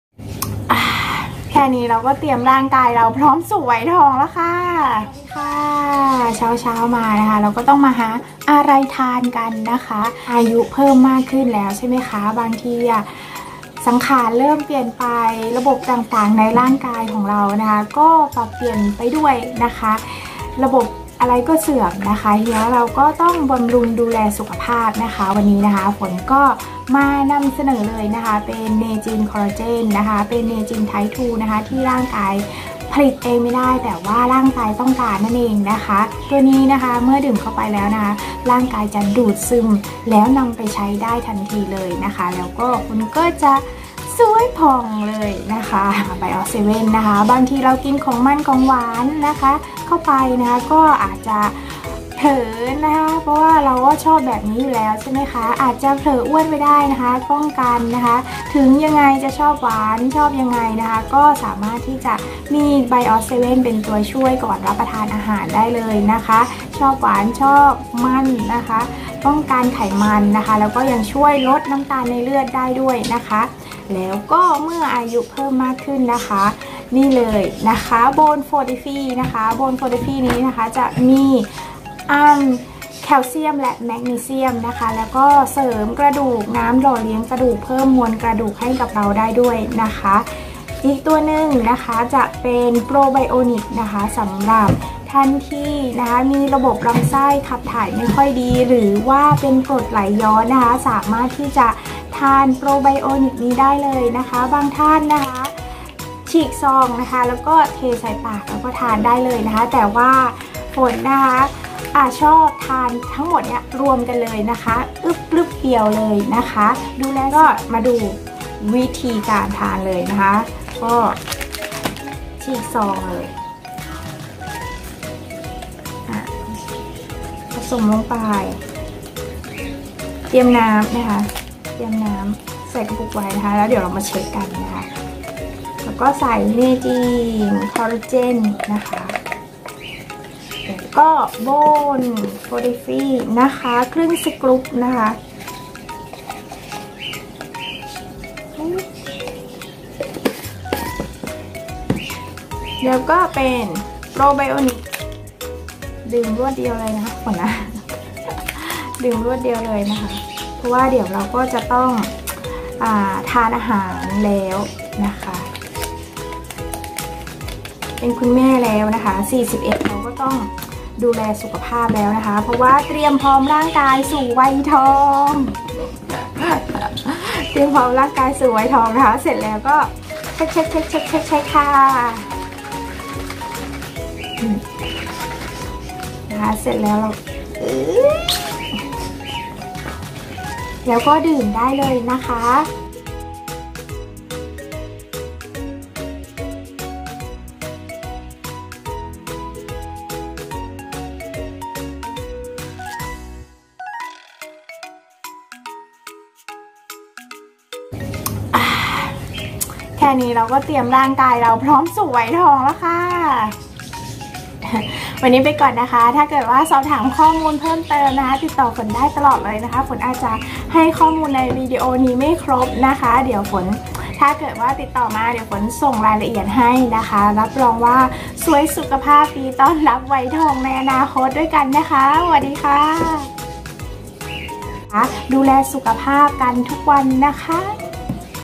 เตรียมพร้อมร่างกายสู่วัยทอง
วัยทอง...นั่นเอง...หนีไม่พ้นนะคะ ทั้งหญิงและชาย 😁

💧เพื่อนๆเตรียมความพร้อมสู่วัยทองยังไงกันบ้าง???